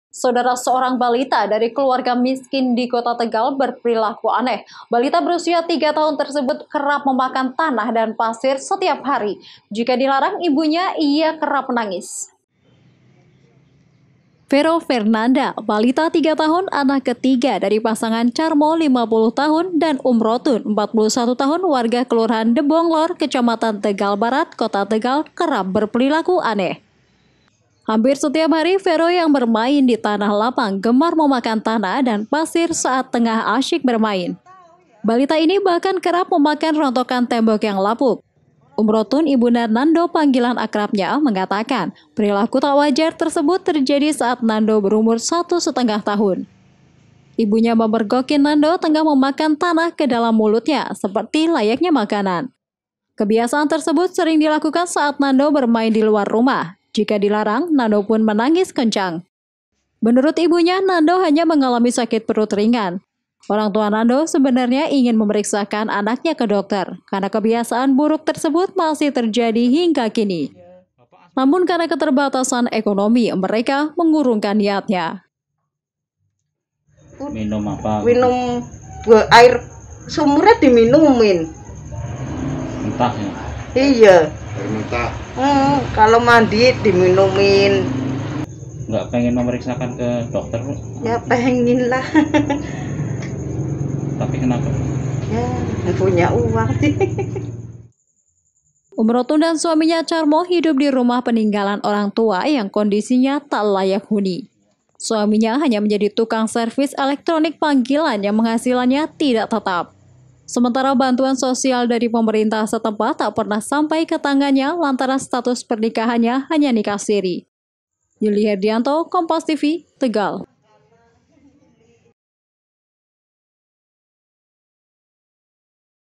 Seorang Balita di Tegal, Gemar Makan Tanah
PEKALONGAN, KOMPAS.TV - Vero Fernanda, balita tiga tahun anak ke tiga dari pasangan Carmo 50 tahun dan Umrotun 41 tahun, warga Kelurahan Debong Lor, Kecamatan Tegal Barat, Kota Tegal, kerap berperilaku aneh. Hampir setiap hari vero yang bermain di tanah lapang gemar memakan tanah dan pasir saat tengah asyik bermain.

Balita ini bahkan kerap memakan rontokan tembok yang lapuk. Umrotun, ibunda Nando panggilan akrabnya mengatakan perilaku tak wajar tersebut terjadi saat Nando berumur satu setengah tahun.

Ibunya memergoki Nando tengah memasukan tanah ke dalam mulutnya, seperti layaknya makanan. Kebiasaan tersebut sering dilakukan saat Nando bermain di luar rumah. Jika dilarang, Nando pun menangis kencang. Menurut ibunya Nando hanya mengalami sakit perut ringan.

Orang tua Nando sebenarnya ingin memeriksakan anaknya ke dokter. Karena kebiasaan buruk tersebut masih terjadi hingga kini. Namun karena keterbatasan ekonomi, mereka mengurungkan niatnya.

Umrotun dan suaminya carmo hidup di rumah peninggalan orang tua yang kondisinya tidak layak huni. Suaminya hanya menjadi tukang servis elektronik panggilan yang penghasilannya tidak tetap. Sementara bantuan sosial dari pemerintah setempat tak pernah sampai ke tangannya lantaran status pernikahannya hanya nikah siri.